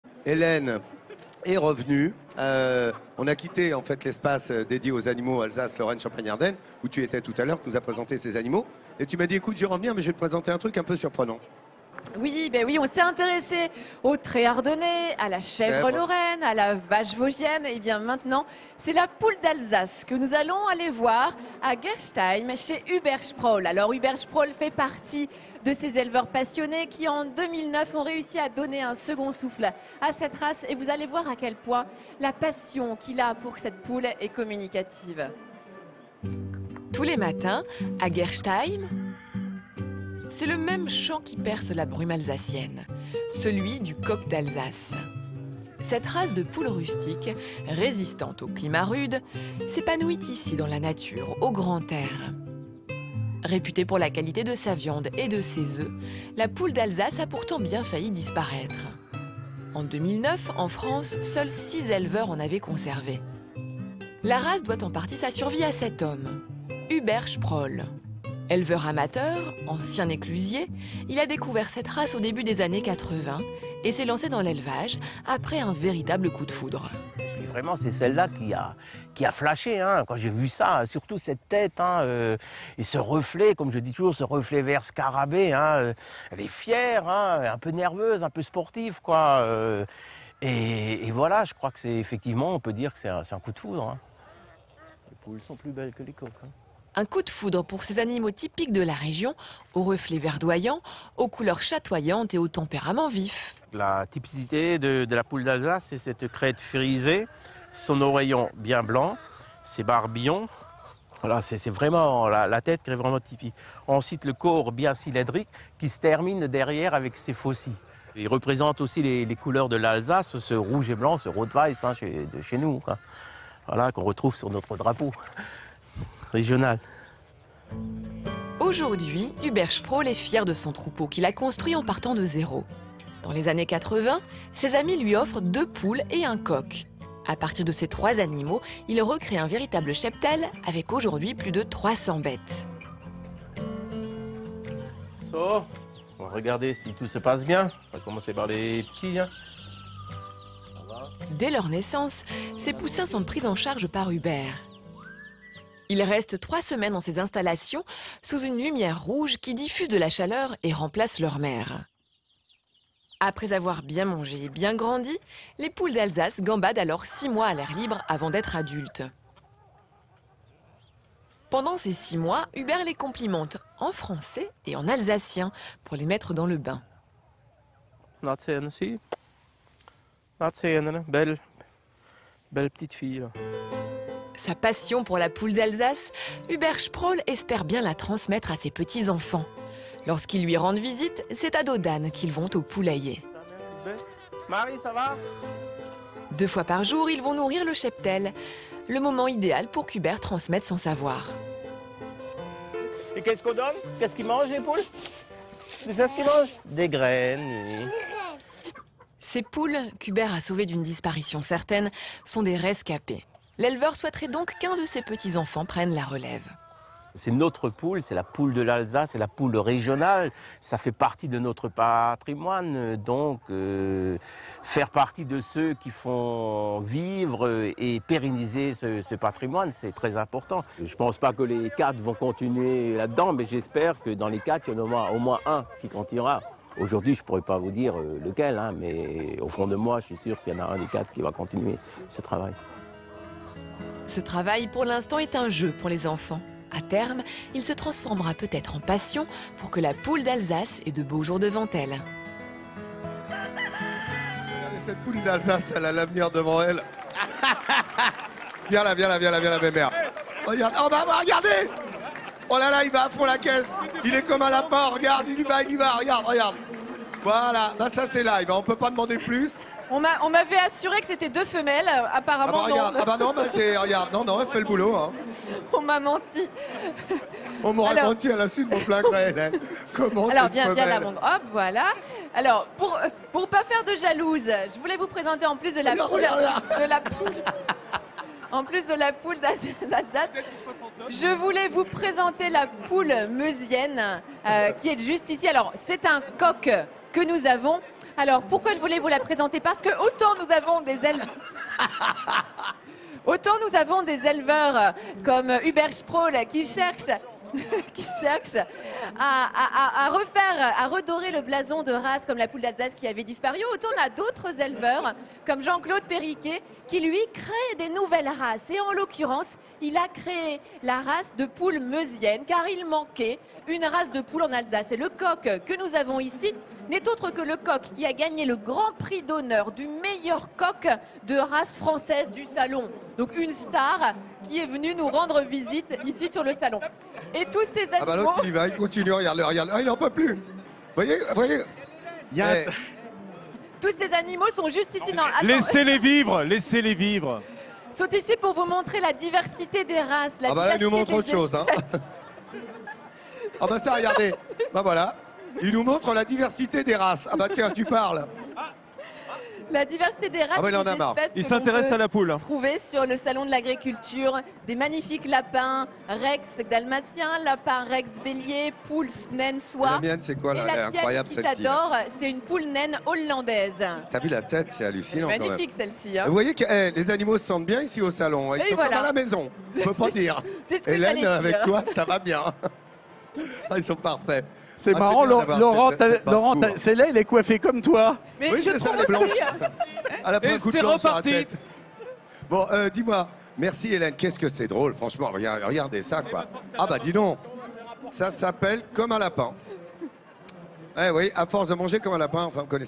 ANIMAUX : la poule d'Alsace et la poule Meusienne
La poule d'Alsace et la poule Meusienne

La poule d’Alsace est appréciée pour la qualité de sa viande et de ses œufs, et pourtant elle a failli disparaître. Hubert a eu un coup de foudre pour cette race au début des années 80. Il en élève en amateur et connaît par cœur leurs spécificités : crête frisée, oreille blanche et les barbillons. Au niveau du corps, elles ont des plumes noires aux reflets verts. Si au début, Hubert n’avait que deux poules et un coq, il en possède aujourd’hui plus de 300. Et comme la poule d’Alsace ne couve pas, il doit utiliser une couveuse pour avoir des poussins. Une fois prêtes, les poules peuvent gambader dans la nature avant d’être, pour les plus belles, présentées à des concours. En 2009, il a gagné le Grand Prix d’honneur des races françaises. Son grand bonheur est de partager cette passion avec ses petits-enfants. Il rêve que l’un d’eux puisse reprendre l’élevage, afin de sauver cette race de poules locale.